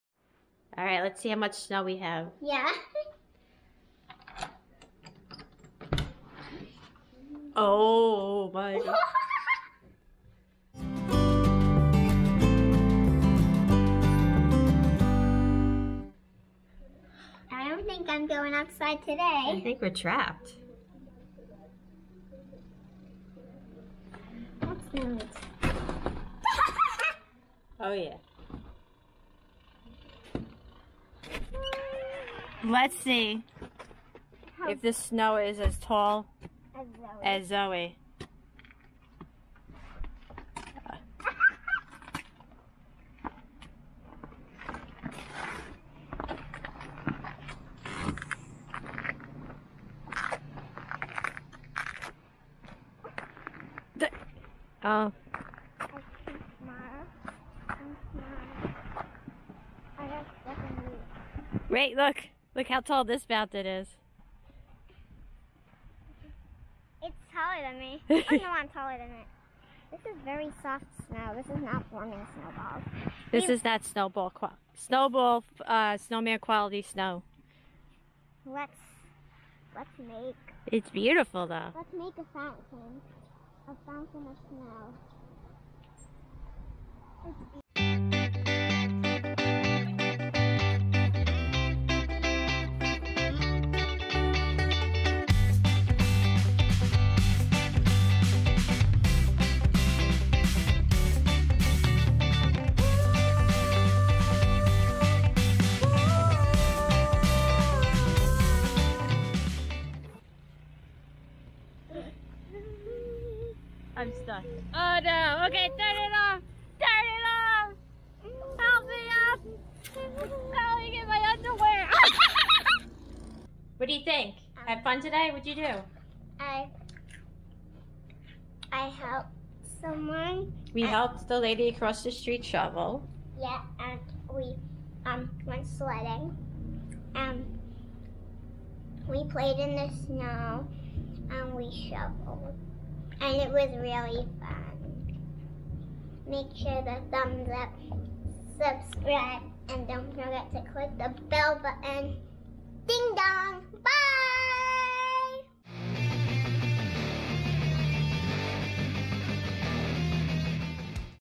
Trapped at Home - Our Fun Day Playing In the Snow - Nor'easter Fun!
The day after the 2022 Nor'easter.  Join us having alot of fun playing out in the snow!